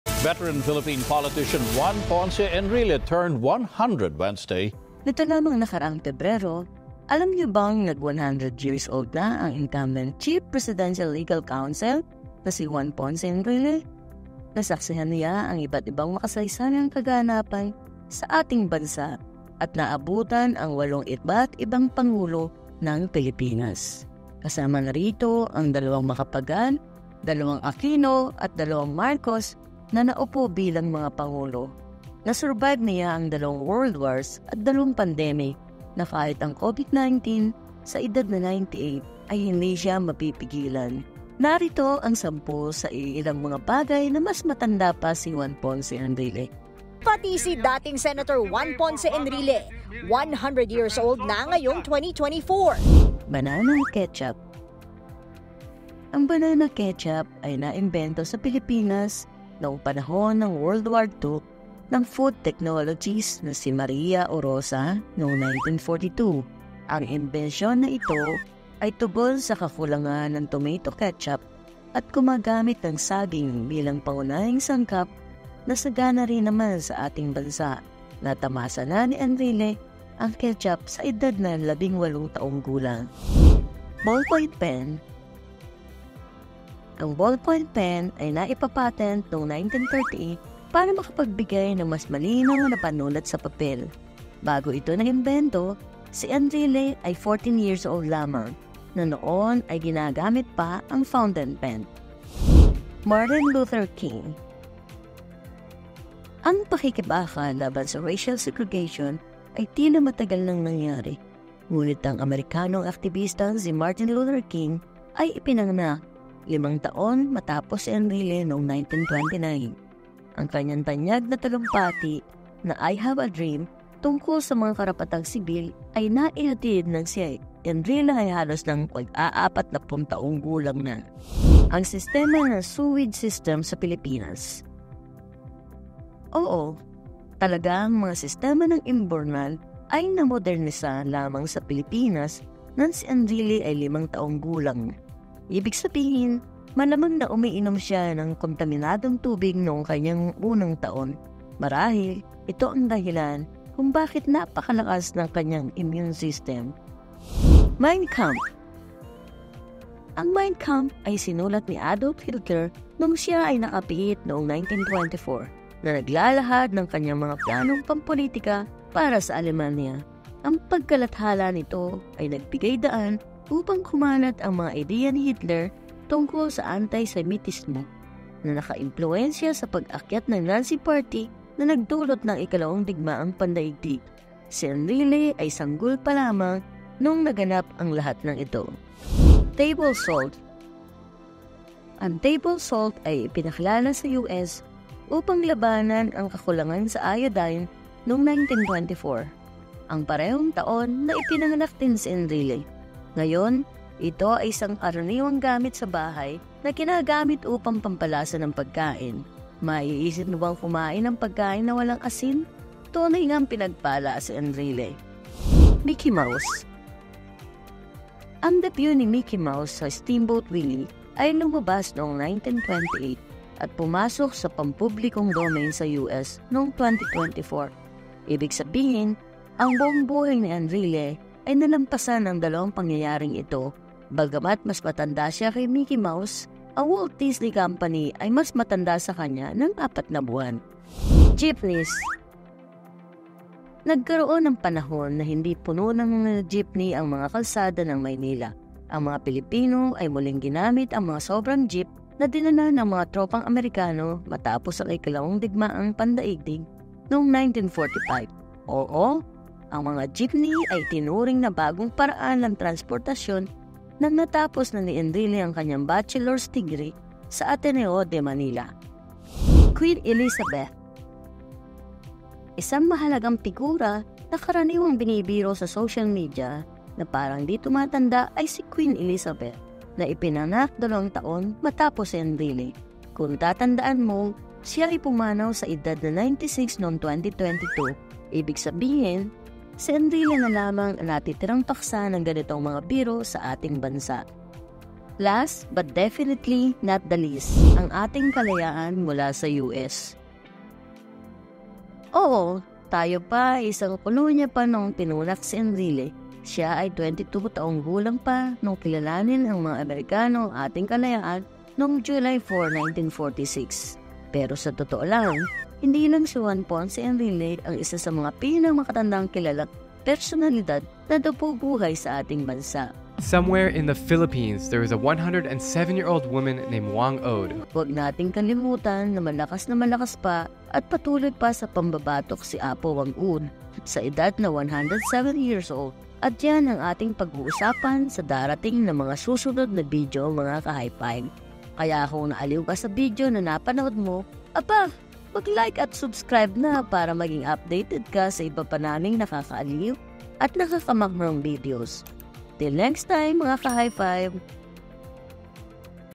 10 Mga Bagay Na Mas Matanda Pa Si Chief Presidential Legal Counsel Juan Ponce Enrile | Centenarian
Neto lamang nakaraang Pebrero, alam niyo bang nag 100 years old na ang incumbent Chief Presidential Legal Counsel na si Juan Ponce Enrile?

Narito ang sampu sa iilang mga bagay na mas matanda pa si Juan Ponce Enrile.

👇🏼You might also like the link below for more Pinoy High Five Videos. 🎥